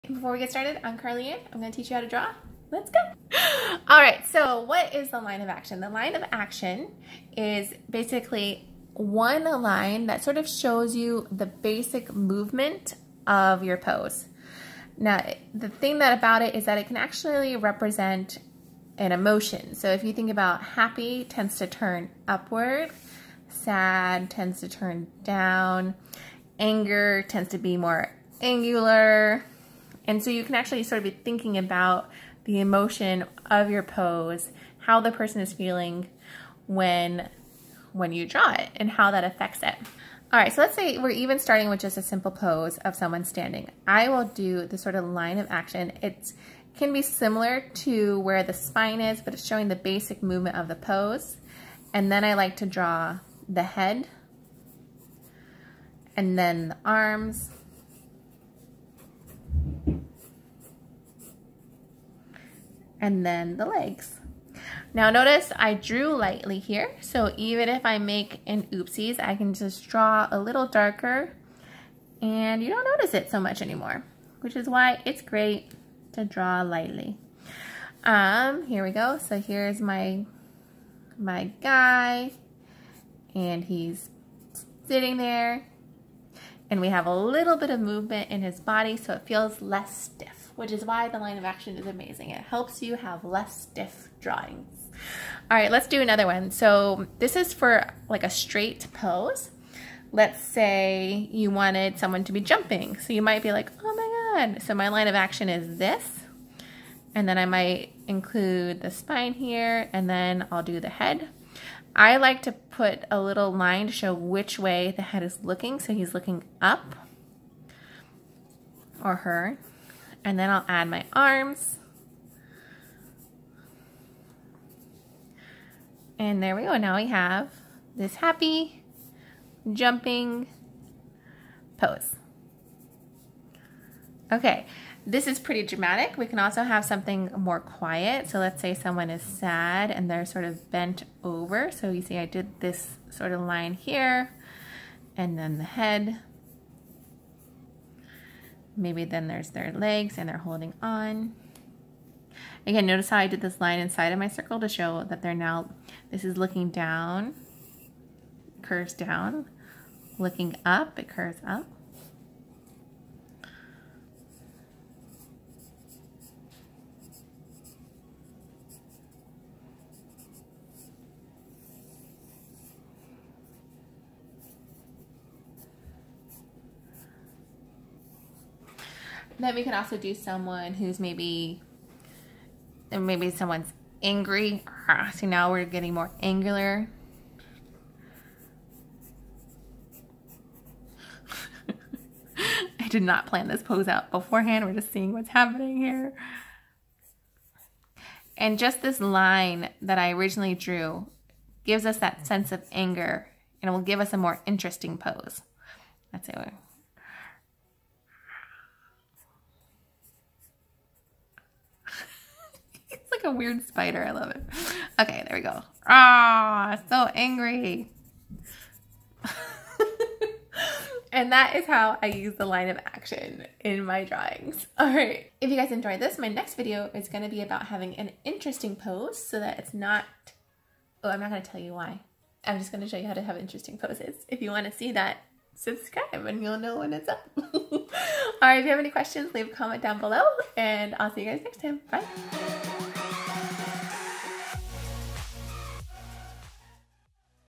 Do this to avoid STIFF poses!
Sorry about drawing along the edge there! Looks like my sketchbook shifted on me.

Okay today I am breaking down how I use the line of action!  This is a simple technique to help you push your poses so that they are more dynamic, and less stiff.  I will be using stick figures today to show that you can do this even if you're an absolute beginner artist.

To see more examples of how I use the line of action, check out my instagram!

Credits
Editing:
Martial Tipsey